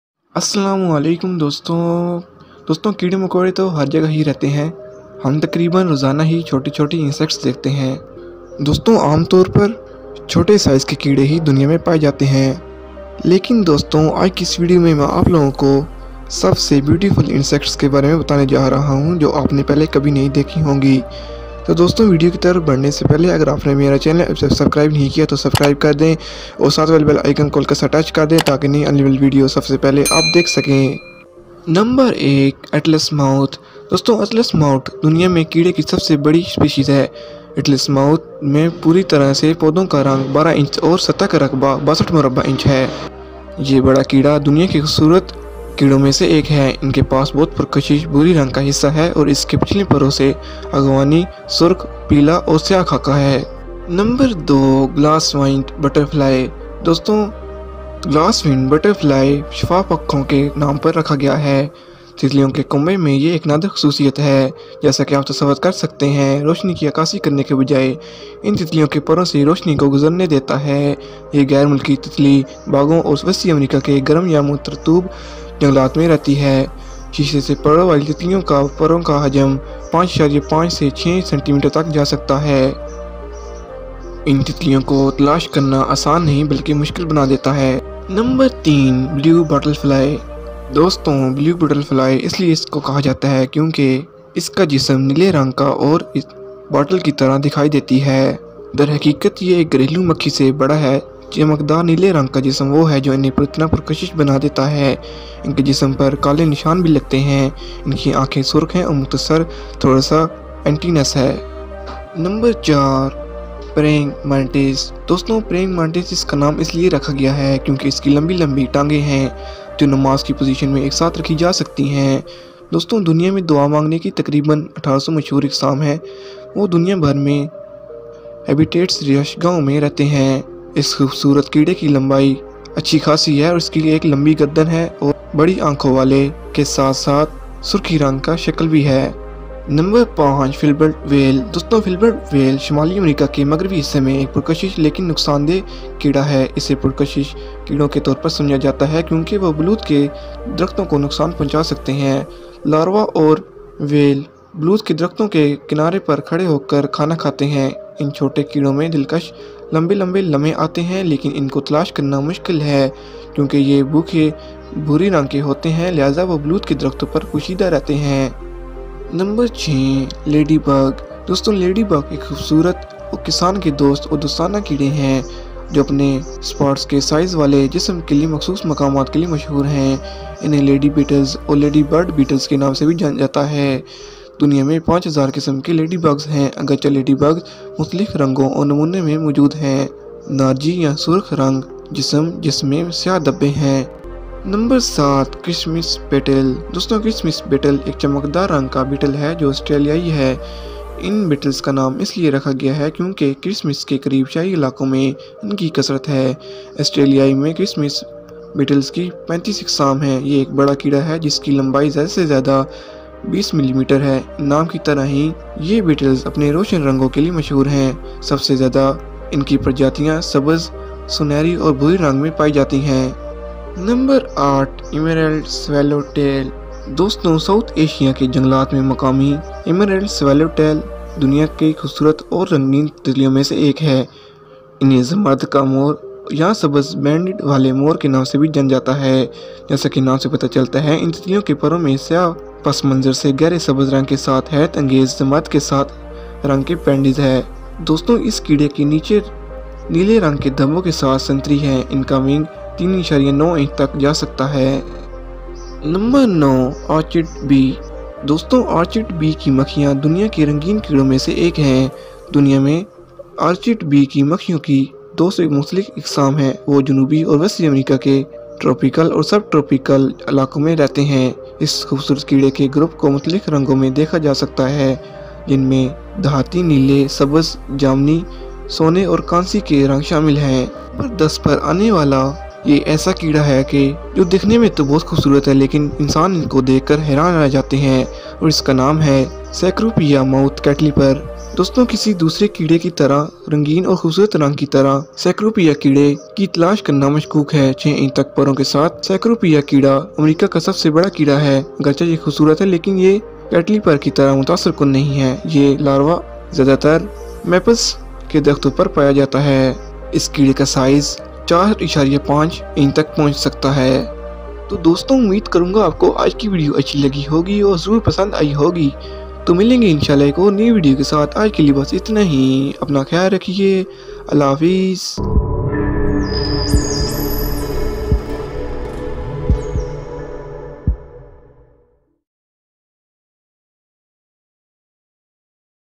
Top 10 Most Beautiful Insects in the World | AbdUllah Tv
Hii Friends..!!!
In this video i will tell u about Top 10 Most Beautiful Insects in the World.
here are following Insects;
1. Atlas Moth
2. Glasswinged Butterfly
3. Blue Bottle Fly
4.  Praying Mantis
5. Filbert Weevil
6. Ladybug
7. Christmas Beetle
8. Emerald Swallowtail
9. Orchid Bee
10. Cecropia Moth Caterpillar
Hope so u like this amazing and informtive video. AbdUllah Tv is an education and entertainment channel dedicated to creating interesting Tops, Lists and more.
Allah Hafiz..